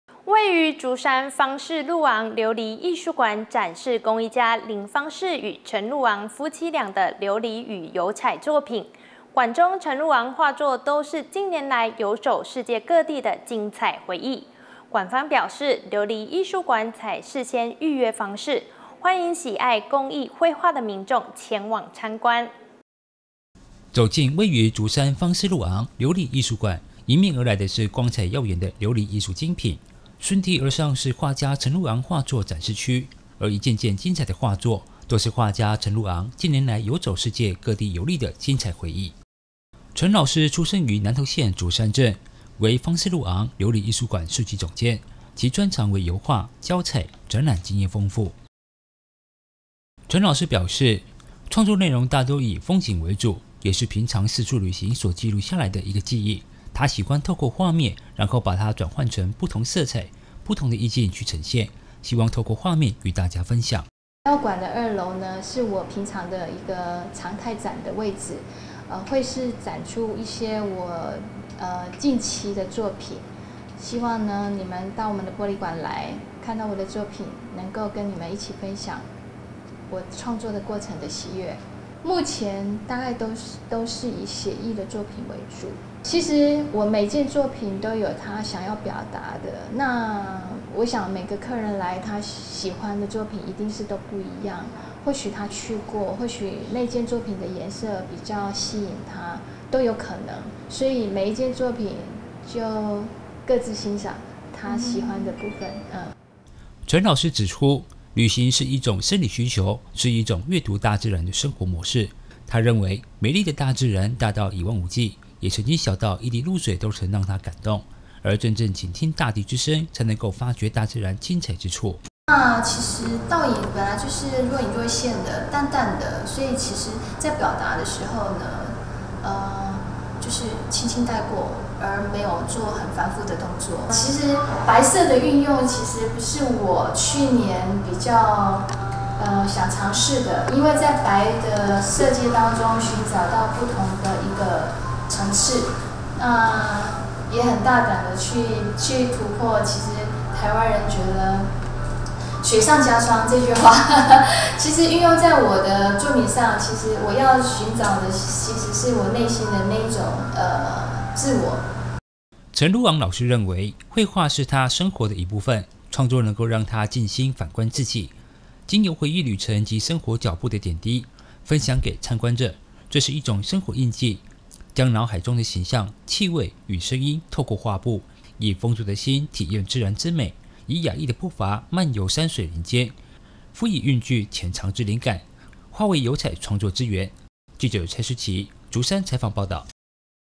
陳璐昂油彩畫作 芳仕璐昂琉璃藝術館展出
位於竹山芳仕璐昂琉璃藝術館，展示工藝家林芳仕與陳璐昂夫妻倆的琉璃與油彩作品，館中陳璐昂的畫作都是近年來遊走世界各地遊歷的精彩回憶。館方表示，琉璃藝術館採事先預約方式參觀，歡迎喜愛工藝畫繪的民眾前往參觀。